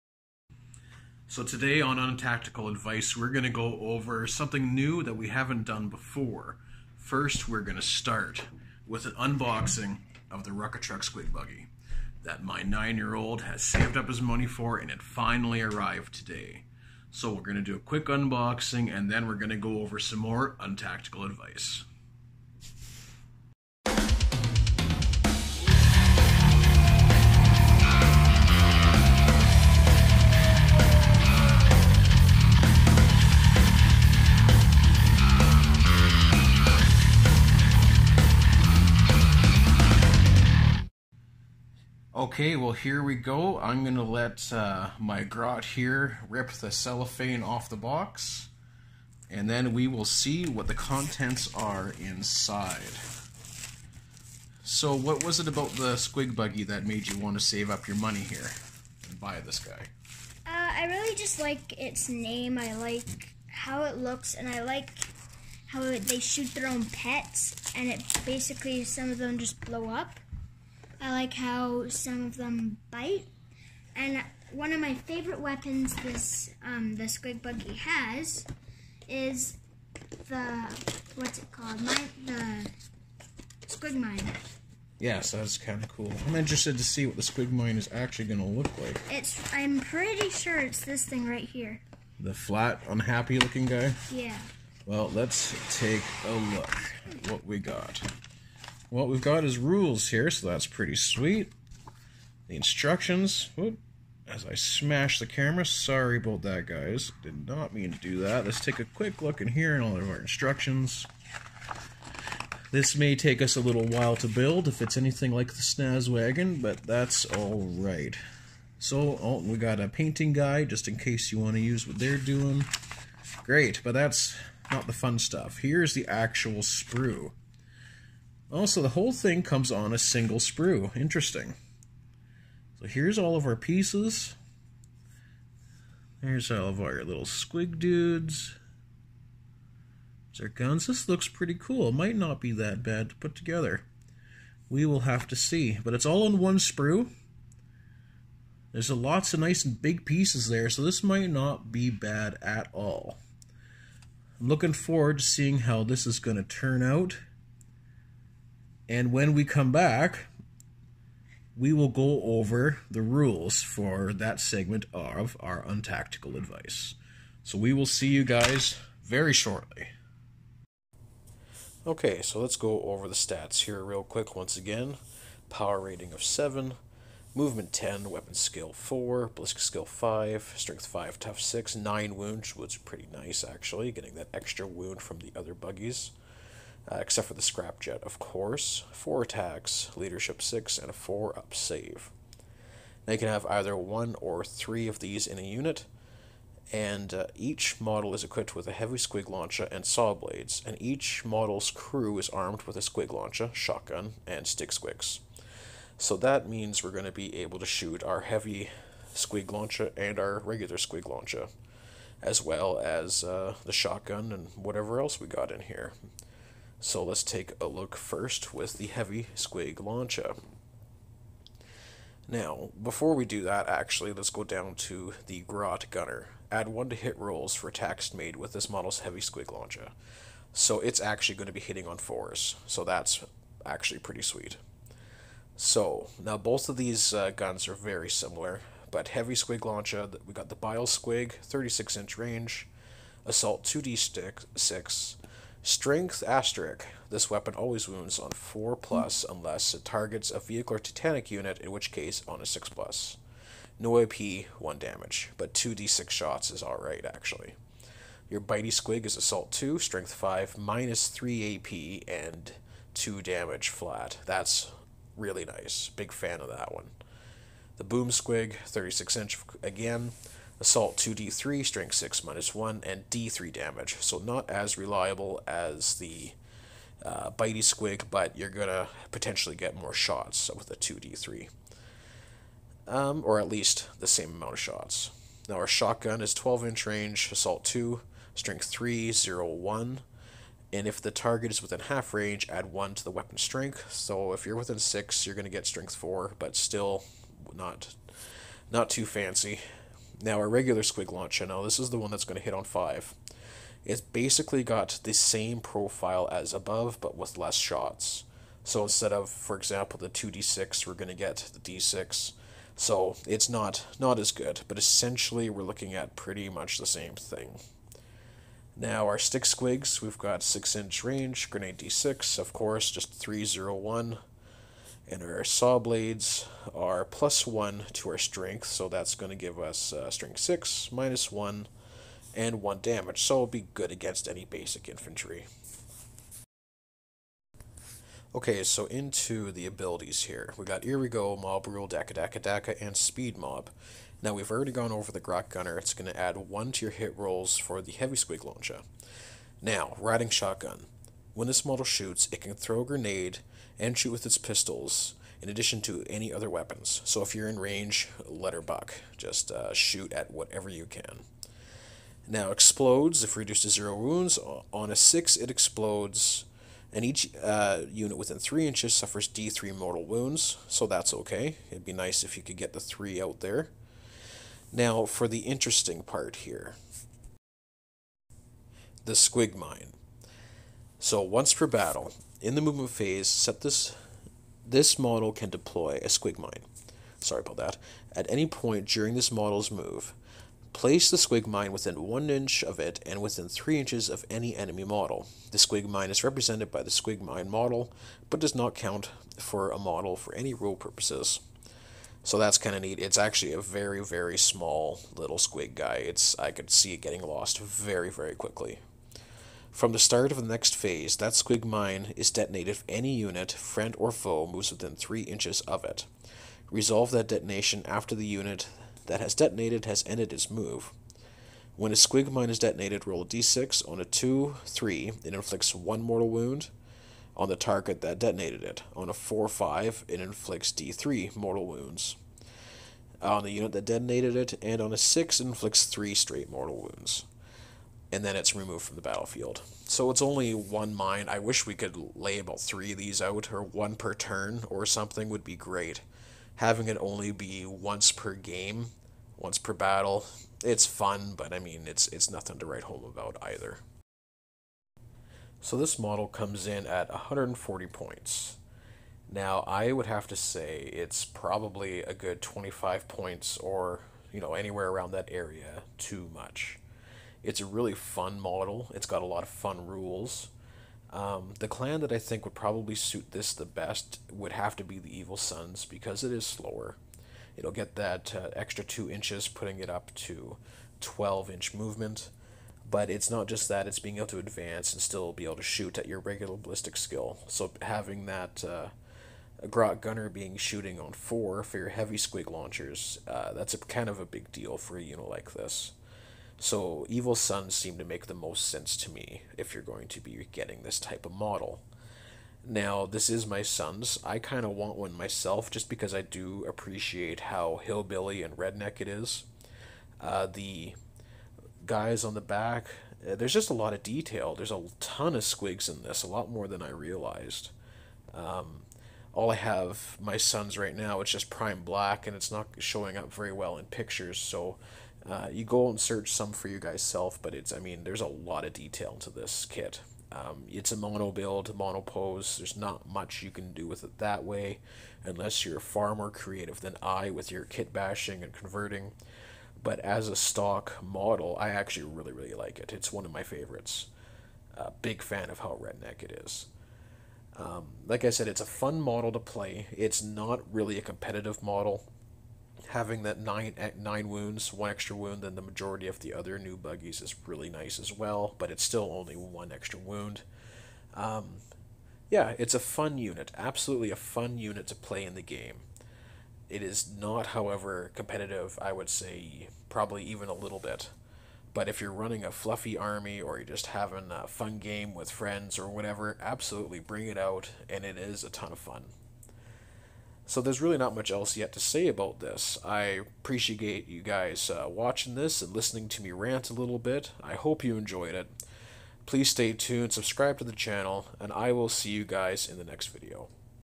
Untaktical Advice: Rukkatrukk Squigbuggy (unboxing)
Let's unbox and talk about the most red neck vehicle that we orks have access to. The Rukkatrukk Squigbuggy.